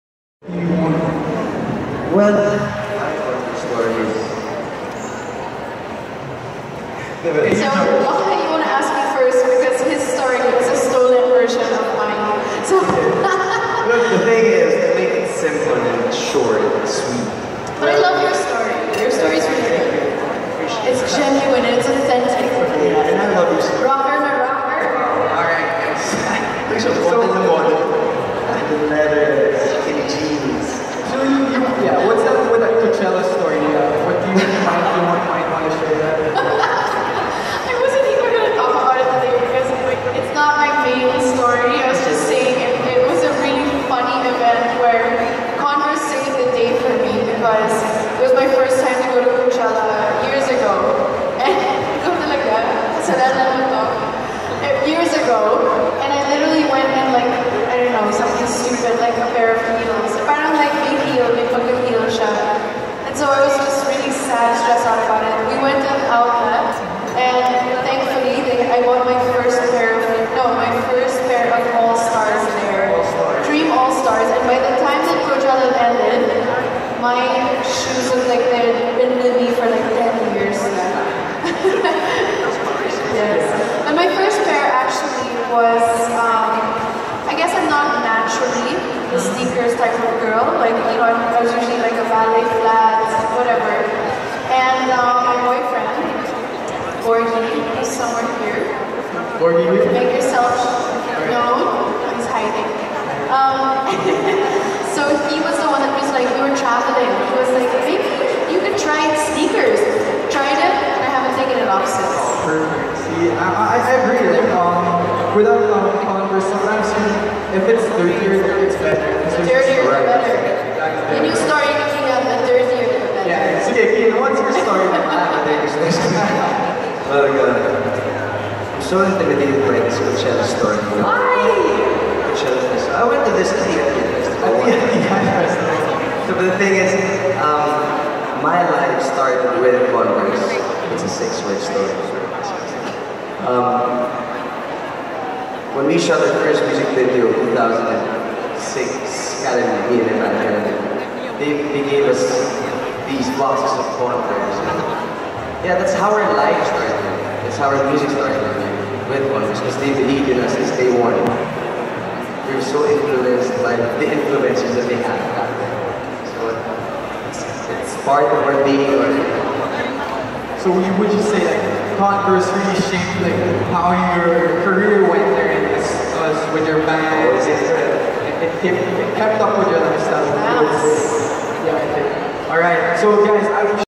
Georgina, Kean at Converse "Made by you"
2 March 2015, The New Glorietta atrium
Georgina Wilson and Kean Cipriano share their Converse story
Shot by Kai Magsanoc